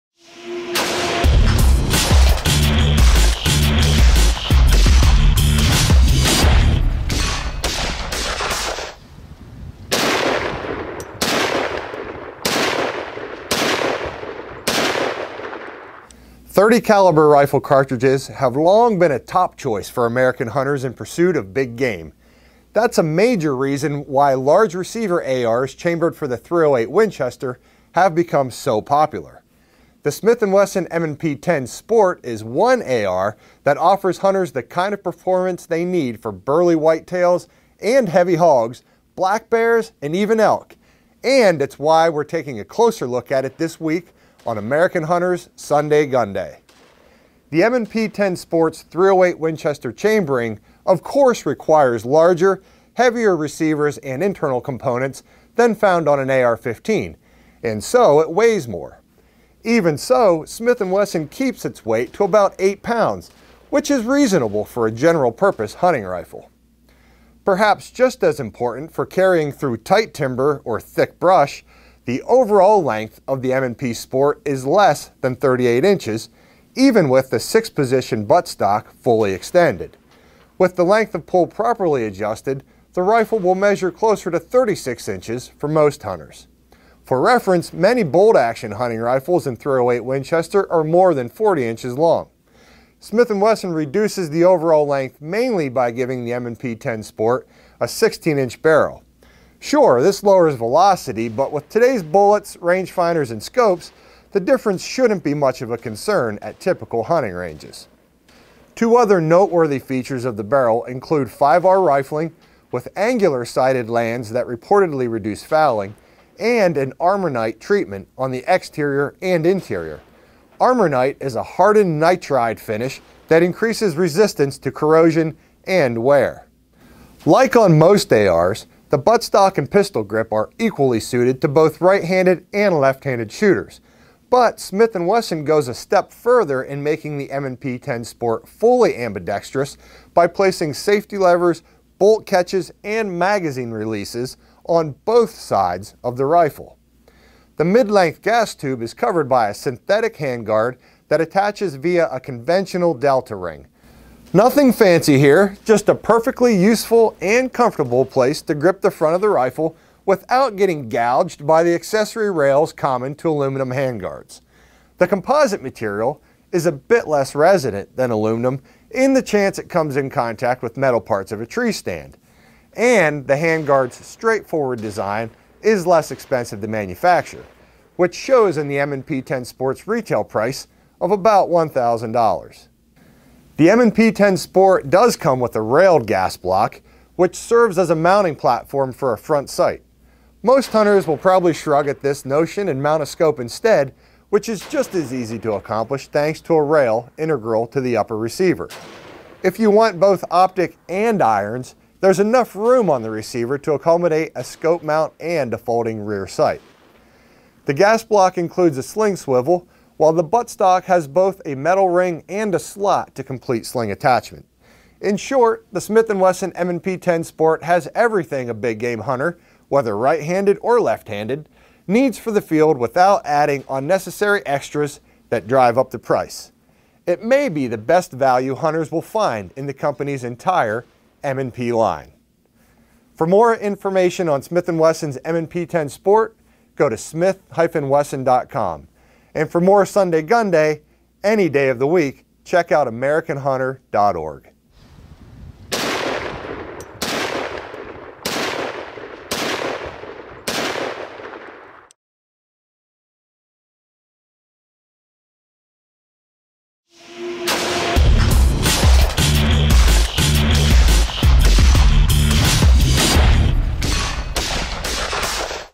SundayGunday: Smith & Wesson M&P10 Sport
Executive Editor Adam Heggenstaller discusses the Smith & Wesson M&P10 Sport, the latest addition to our SundayGunday series.